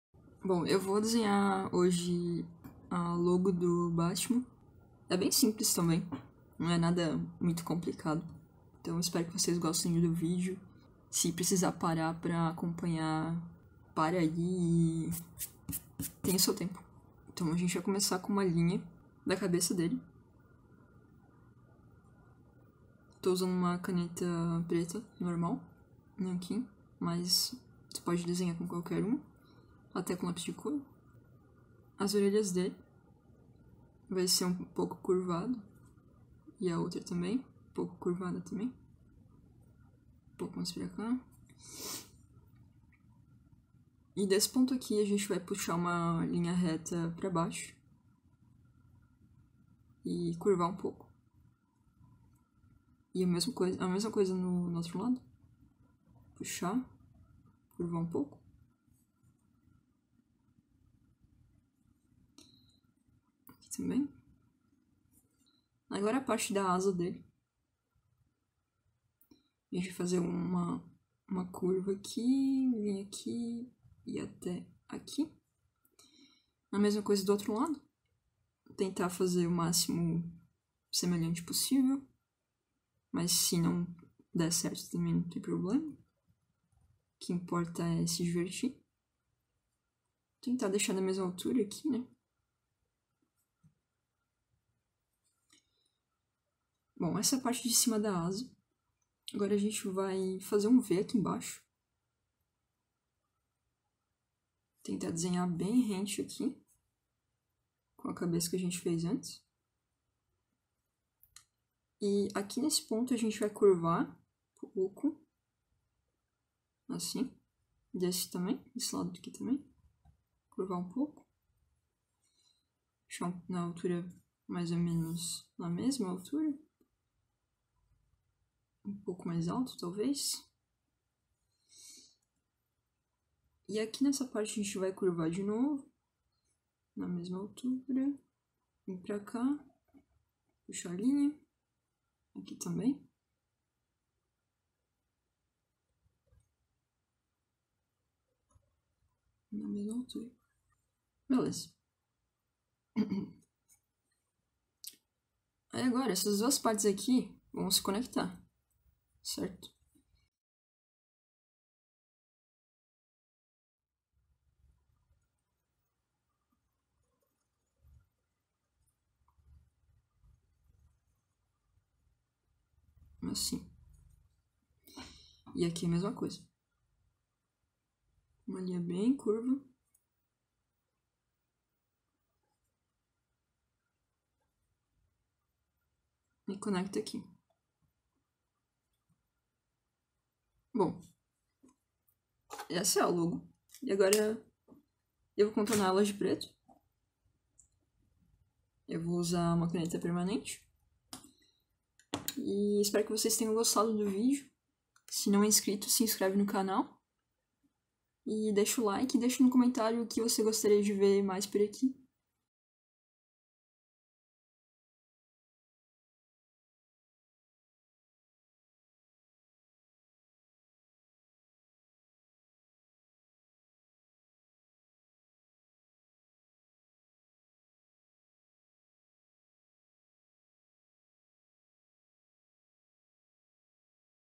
Como desenhar a Logo do Batman | Passo a Passo
Oi galera, ta aí um desenho mais fácil pra vocês, espero que gostem!
Tem videos novos todos os dias aqui no canal. 😃😜
Ah, e deixa nos comentários aí o que você gostaria de ver mais por aqui!